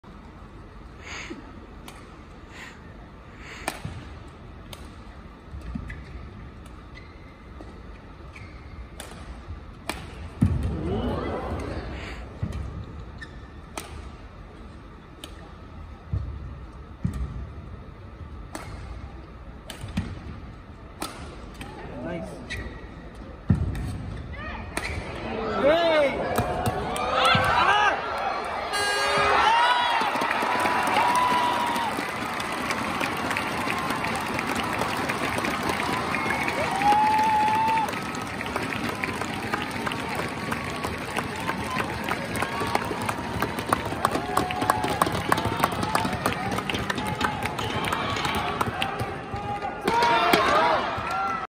Uber cup Final Best Rally (An se young vs Chen Yufei)
결과야 어쩔 수 없겠지만 충분히 즐거운 시간을 만들어준 두 선수에게 감사함을 전한다.